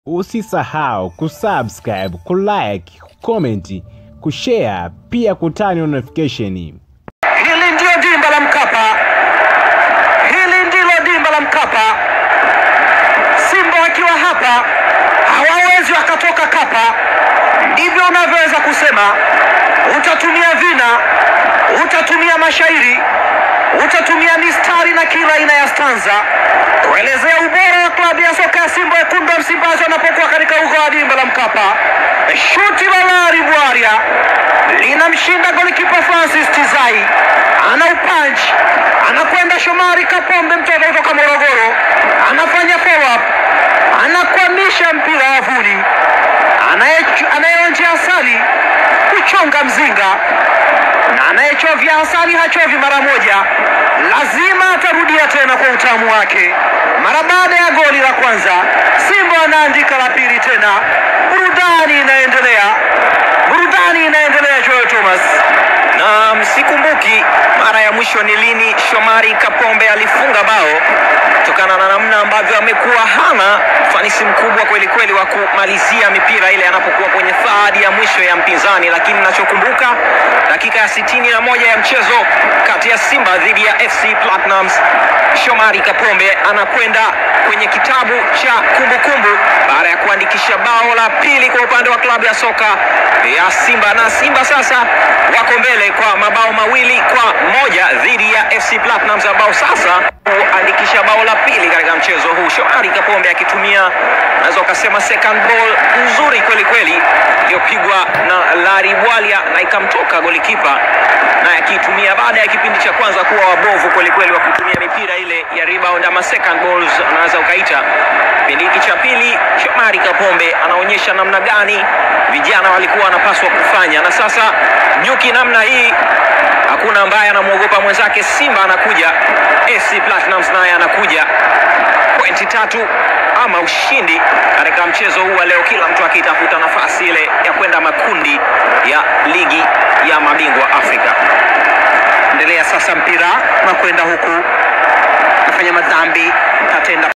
Goli la 2/ Simba SC VS FC Platinum/ Goli la Shomali Kapombe
GOLI LA PILI LA SIMBA SC
GOLI LA SHOMALI KAPOMBE
SIMBA SC 2 - 0 FC PLATINUM
AGGREGATE
SIMBA SC 2 - 1 FC PLATINUM